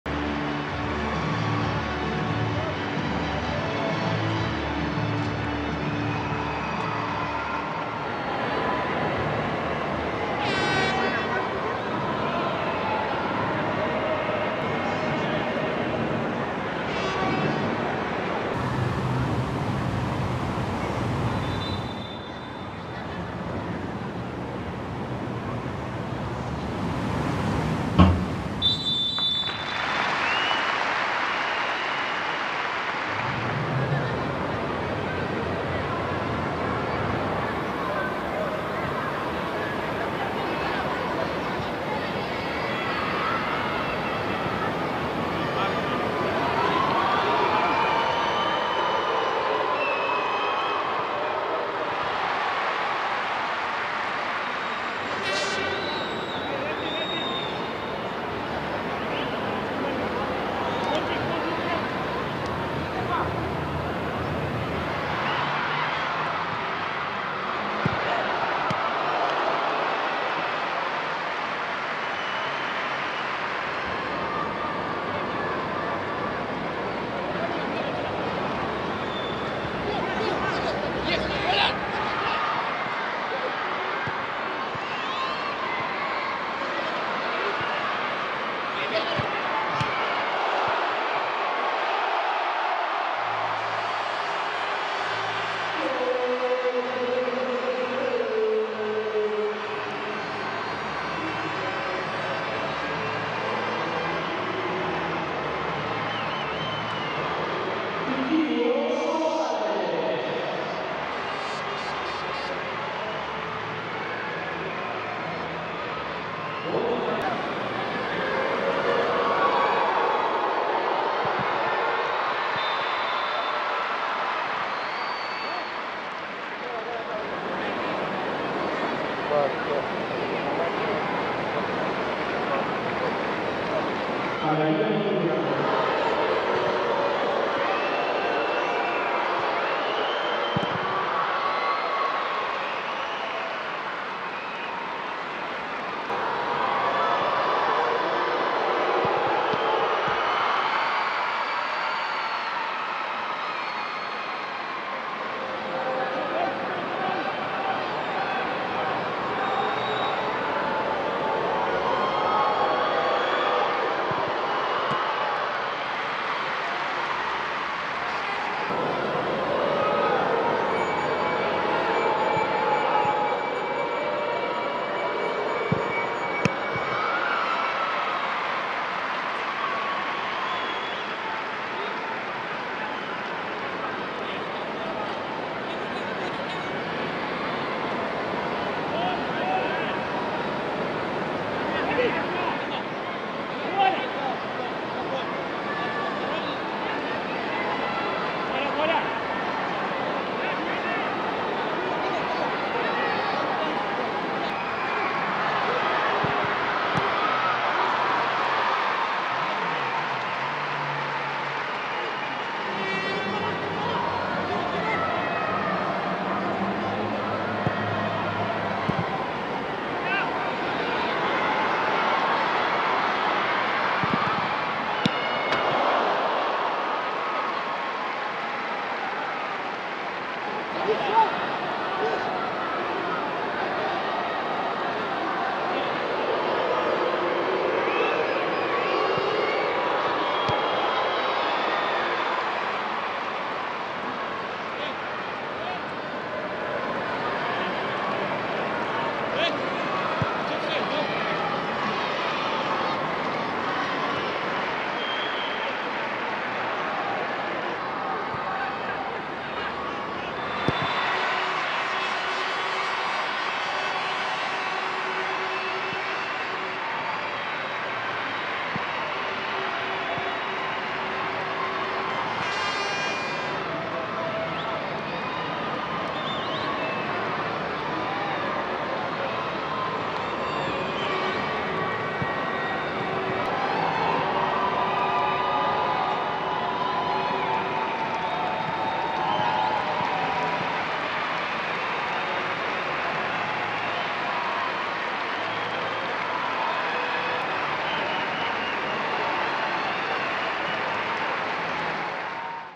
За мир в Україні! Олімпіакос – Шахтар – 1:0. Огляд матчу Shakhtar Global Tour (09.04.2022)
9 квітня «Шахтар» у Афінах провів благодійний матч з «Олімпіакосом» у межах Shakhtar Global Tour for Peace.

Усі кошти від реалізації квитків та реклами буде передано захисникам України, волонтерським організаціям, медикам, а також спрямовано на допомогу українським дітям, які постраждали від війни.